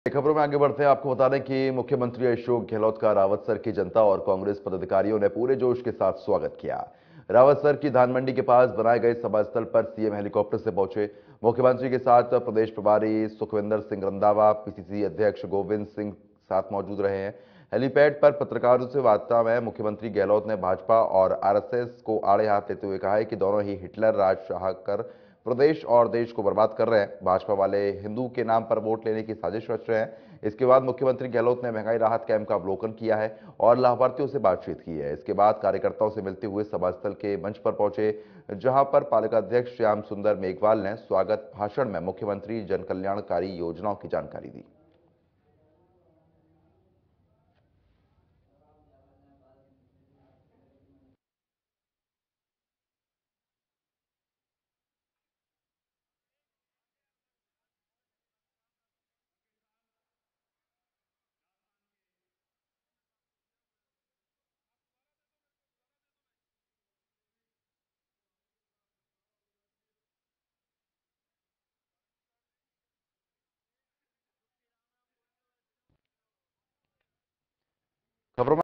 Rawatsar News : रावतसर पहुंचे मुख्यमंत्री अशोक गहलोत | Hindi News
Rawatsar News : रावतसर पहुंचे मुख्यमंत्री अशोक गहलोत,पदाधिकारियों ने जोश के साथ किया स्वागत | Hindi News

मुख्यमंत्री अशोक गहलोत का रावतसर की जनता और कोंग्रेस पदाधिकारीयों ने पूरे जोश के साथ स्वागत किया, रावतसर की धान मंडी के पास बनाये गए सभा स्थल पर सीएम हेलीकॉप्टर से पहुंचे, मुख्यमंत्री के साथ प्रदेश प्रभारी सुखविंदर सिंह रन्धावा, पीसीसी अध्यक्ष गोविंद सिंह भी साथ में मौजूद रहे, हेलीपेड पर पत्रकारों से वार्ता में मुख्यमंत्री गहलोत ने भाजपा और आरएसएस को आड़े हाथों लेते कहा कि,दोनों ही हिटलर शाही राज कर प्रदेश और देश बर्बाद कर रहे,भाजपा वाले हिन्दू के नाम पर वोट लेने की साजिश रच रहे हैं .